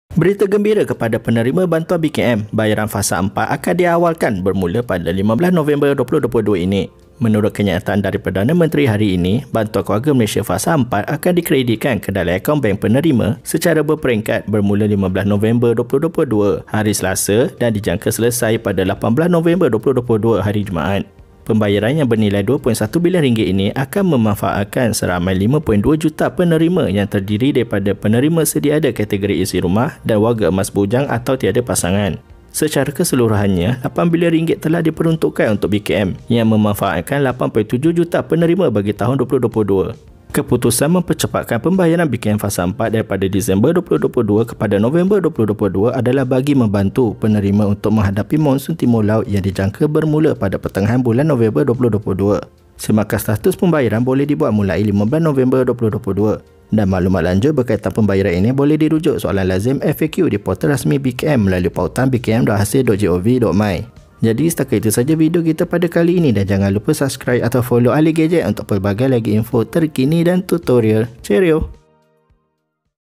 Tarikh Bayaran Bantuan BKM Fasa 4 Bermula 15 November 2022 Secara Berperingkat
Menurut kenyataan dari Perdana Menteri hari ini, Bantuan Keluarga Malaysia Fasa 4 akan dikreditkan ke dalam akaun bank penerima secara berperingkat bermula 15 November 2022 hari Selasa dan dijangka selesai pada 18 November 2022 hari Jumaat.

Pembayaran yang bernilai RM2.1 bilion ini akan memanfaatkan seramai 5.2 juta penerima yang terdiri daripada penerima sedia ada kategori isi rumah dan warga emas bujang/tiada pasangan. Secara keseluruhannya, RM8 bilion telah diperuntukkan untuk BKM yang memanfaatkan 8.7 juta penerima bagi tahun 2022.

Keputusan mempercepatkan pembayaran BKM Fasa 4 daripada Disember 2022 kepada November 2022 adalah bagi membantu penerima untuk menghadapi Monsun Timur Laut yang dijangka bermula pada pertengahan bulan November 2022.

Semakan status pembayaran boleh dibuat mulai 15 November 2022 dan maklumat lanjut berkaitan pembayaran ini boleh dirujuk Soalan Lazim (FAQ) di Portal Rasmi BKM melalui pautan